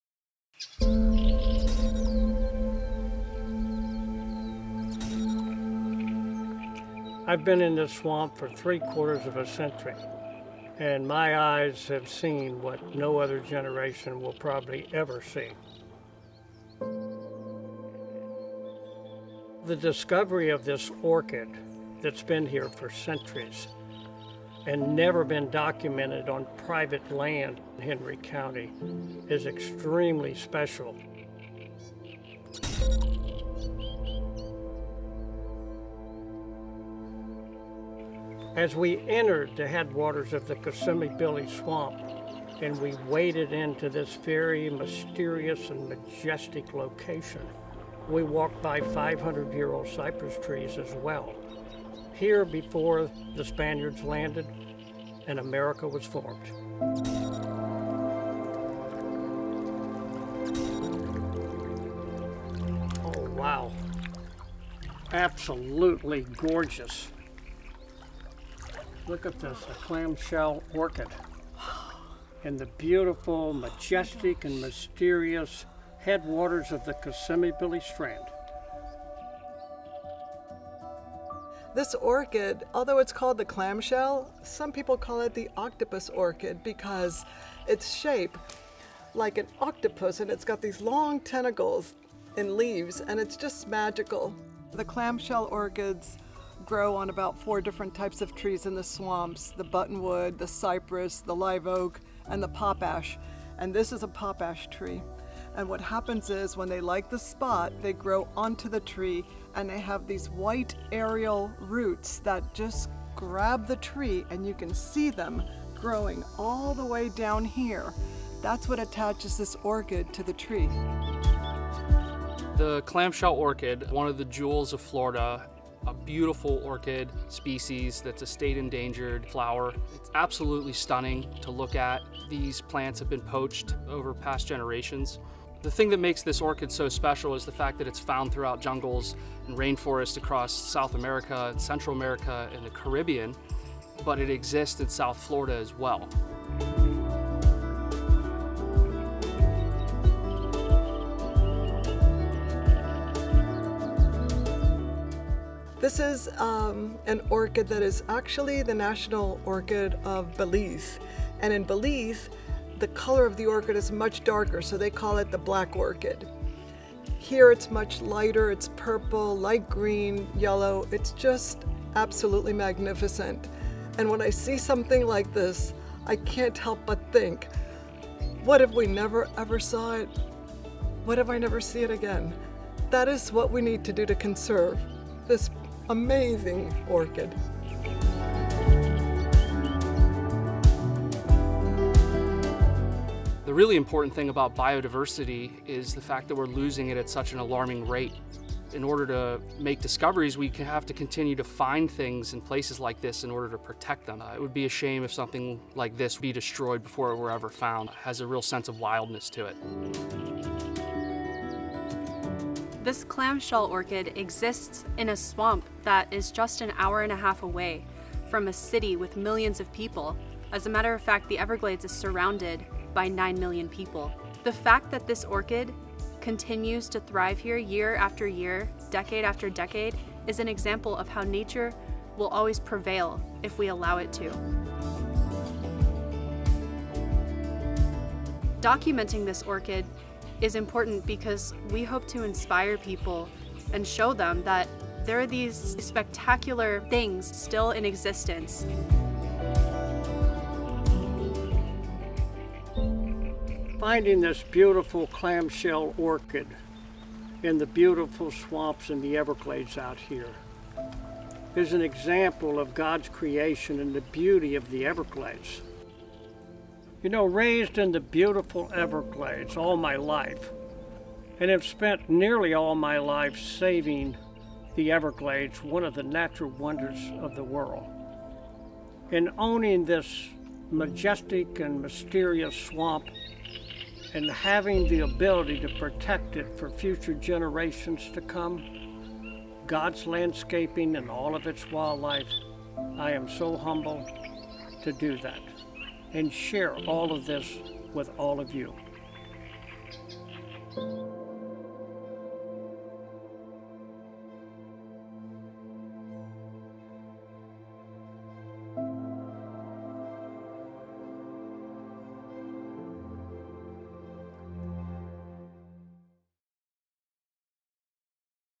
RARE and ENDANGERED ORCHID DISCOVERY in the SWAMPS
NEW DISCOVERIES happen more often than you think! Vine snakes aren't the only species being discovered right now. Join us in the swamps at Green Glades West to see our latest SCIENTIFIC DISCOVERY!

The RARE and ENDANGERED Clamshell Orchid has never been found in Hendry County or on private land... until now!